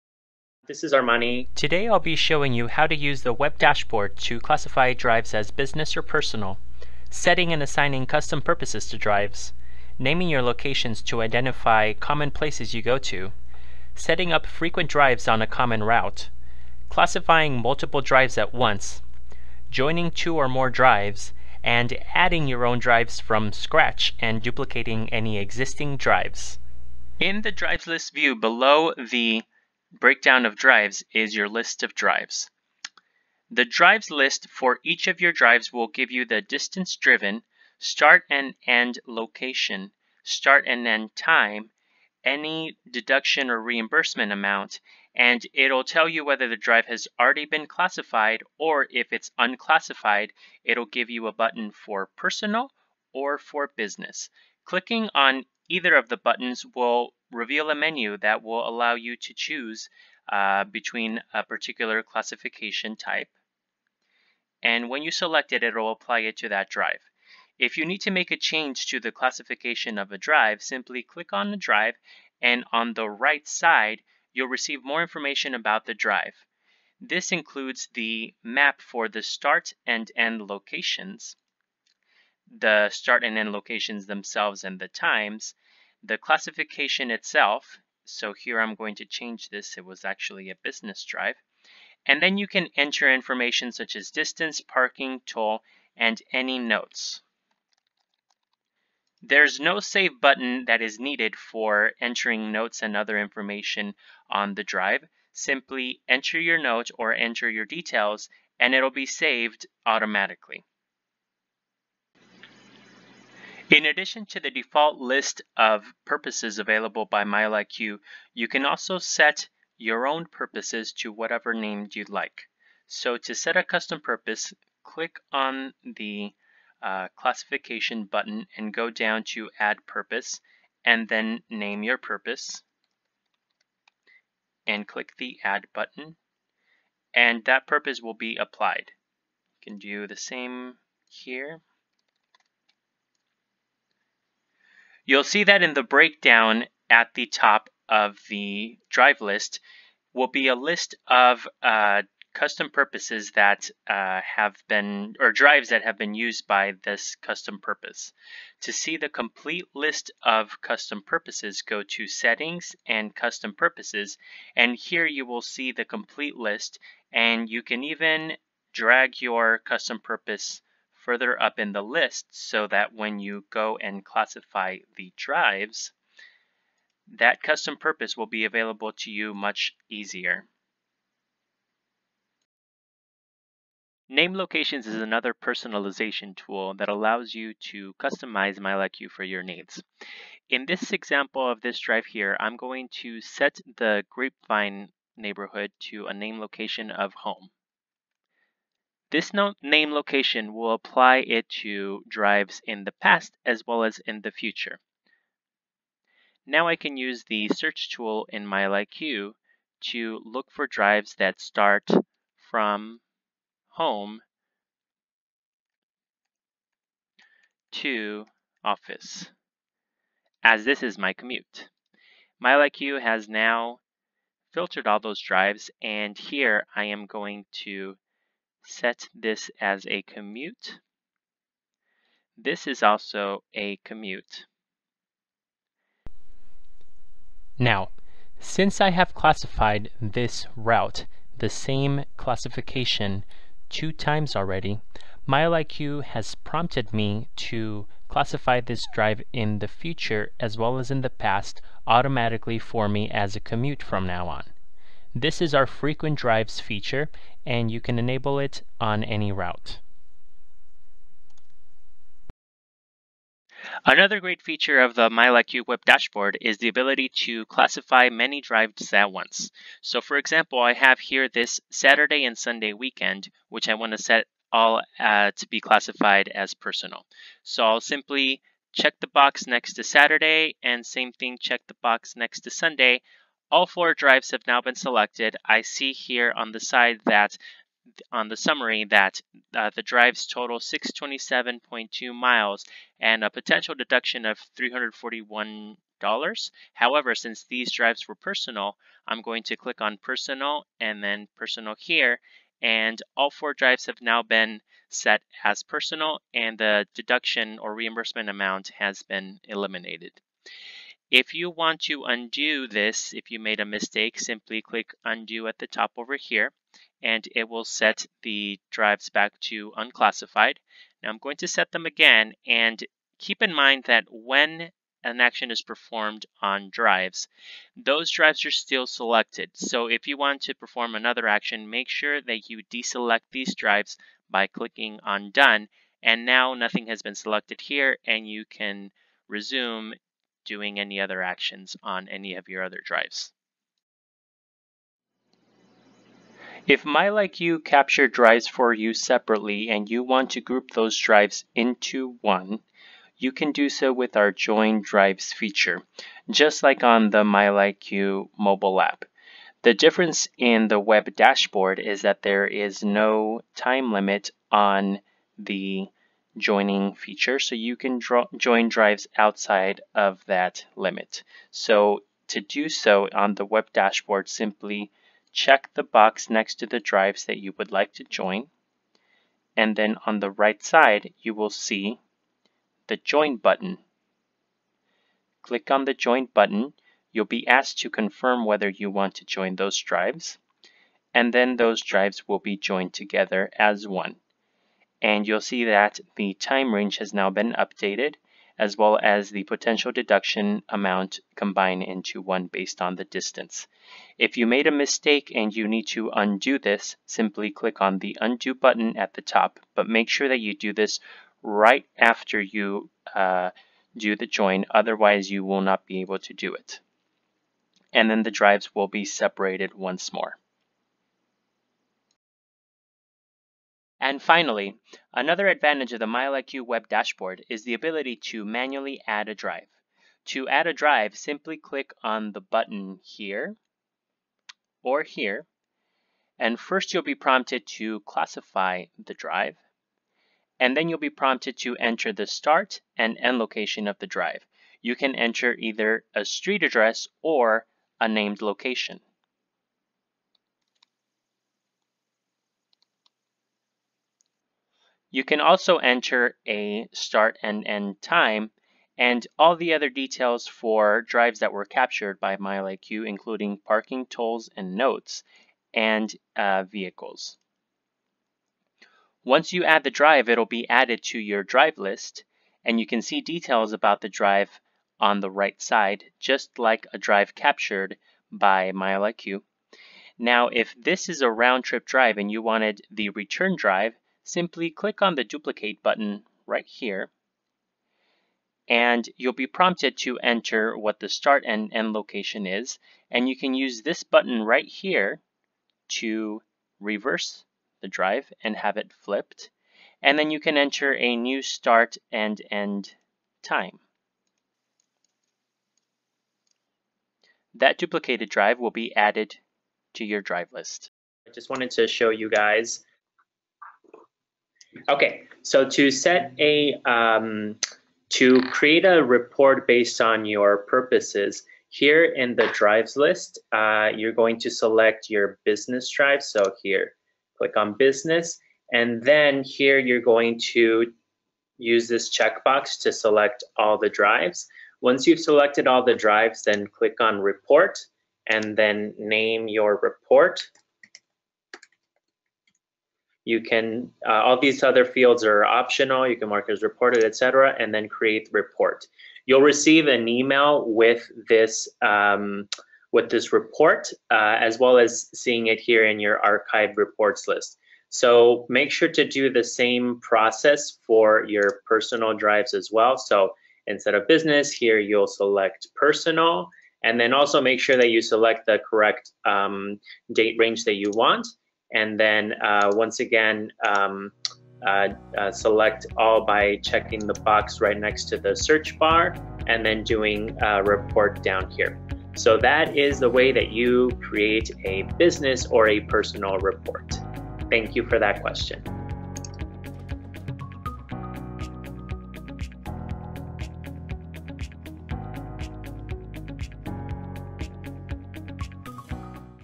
MileIQ Webinar - How to: Classify with the Web Dashboard
MileIQ is the leading automatic mileage tracking app for iPhone and Android. With automatic mileage tracking and easy reporting, MileIQ is the easiest way to create a mileage log for taxes or reimbursements.

This segment from "Classification Tips & Tricks: A MileIQ Webinar" covers:
• Use Web Dashboard to classify drives
• Set and assign custom purposes
• Apply Named Locations to common locations
• Setting up Frequent Drives
• Classifying multiple drives at once
• Joining drives
• Adding & duplicating drives